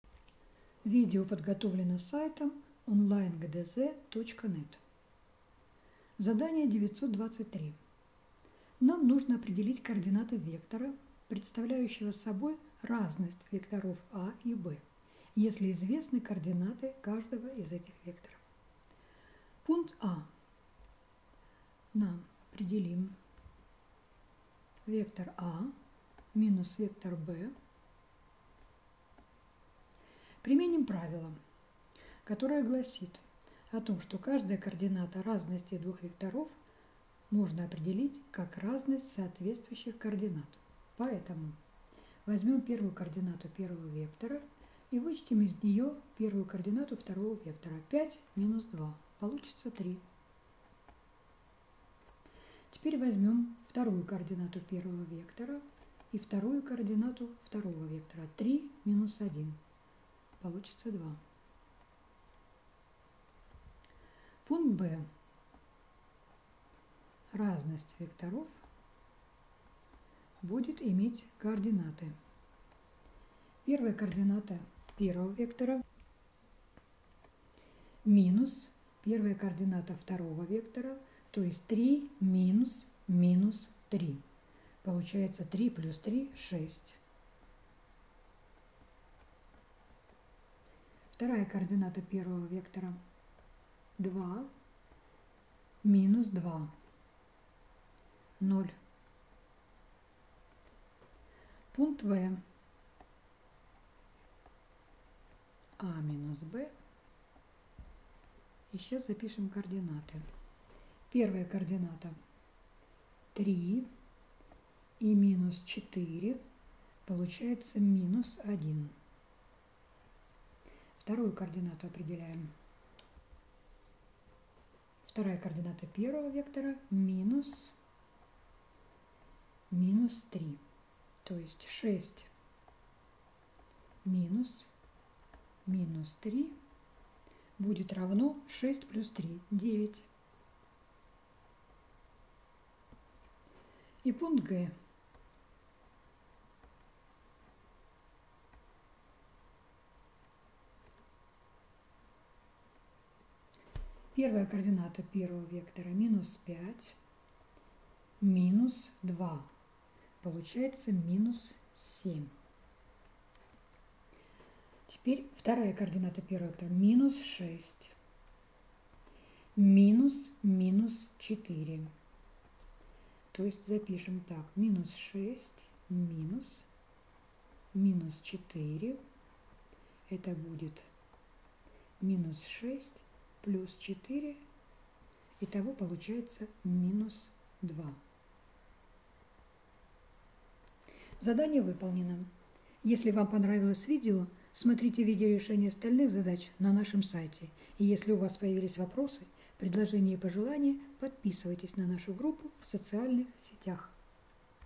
Задание № 923 — Геометрия 9 класс (Атанасян)
Для решения задания 923 нужно знать правило определения координат разности двух векторов. Оно записано в 1 параграфе 10 главы учебника.